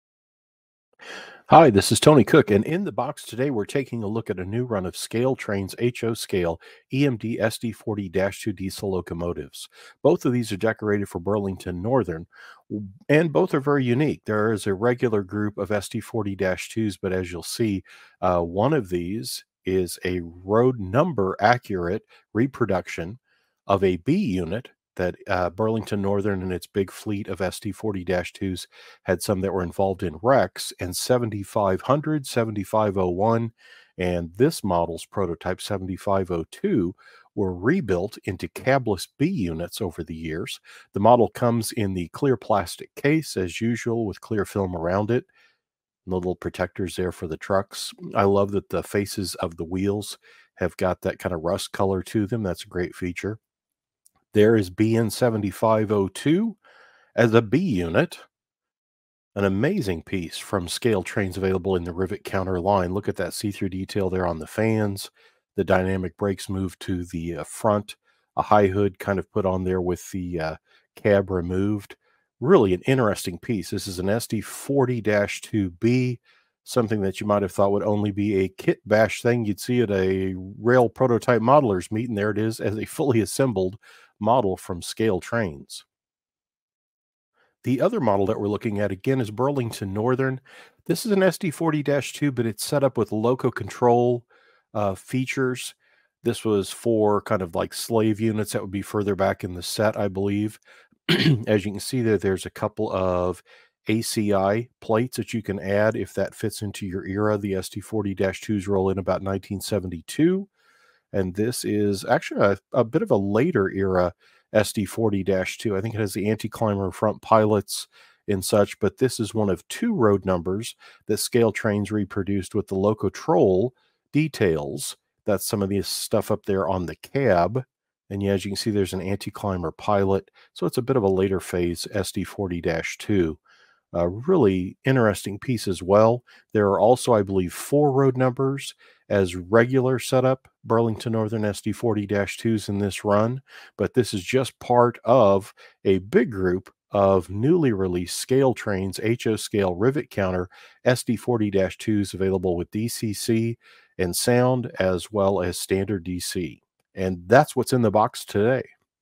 ScaleTrains HO BN SD40-2 and SD40-2B - Whats In The Box
An unboxing video showing ScaleTrains HO-scale SD40-2 and SD40-2B diesel locomotive models decorated for Burlington Northern from a spring 2024 delivery.